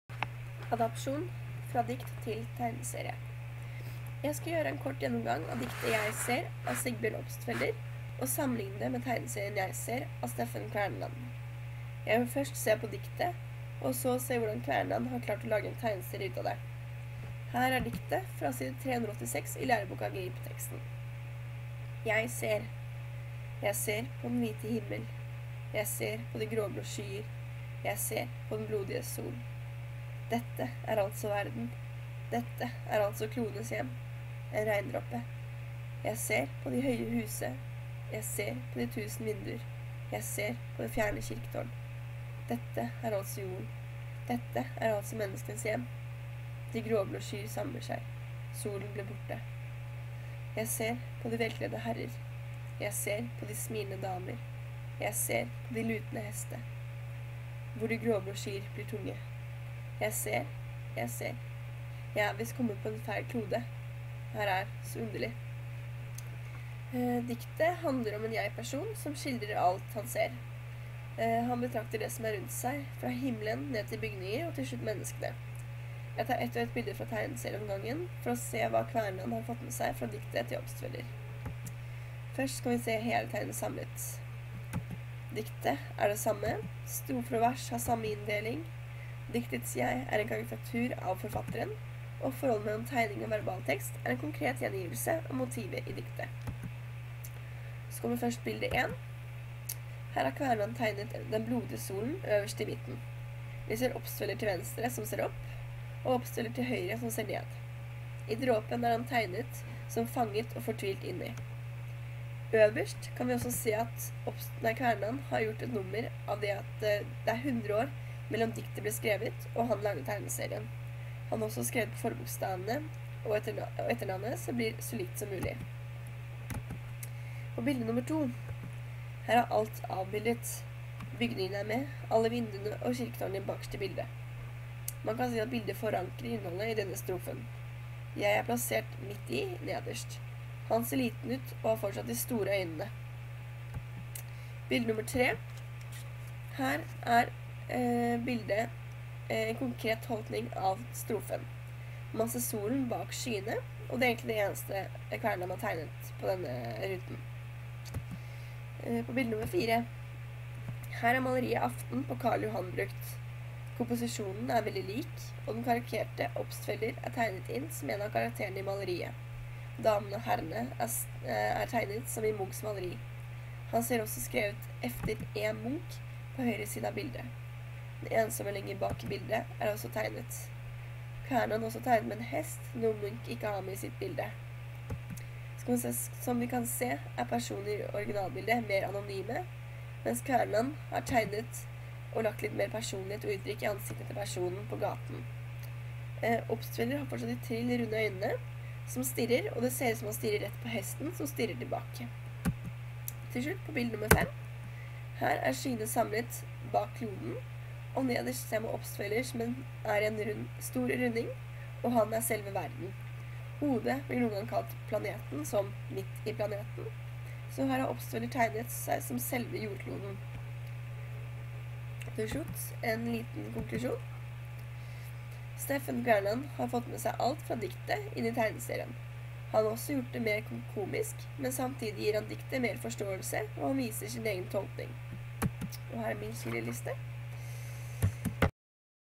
Adapsjon - fra dikt til tegneserie
"Jeg ser" av Sigbjørn Obstfelder og "Jeg ser" av Steffen Kverneland.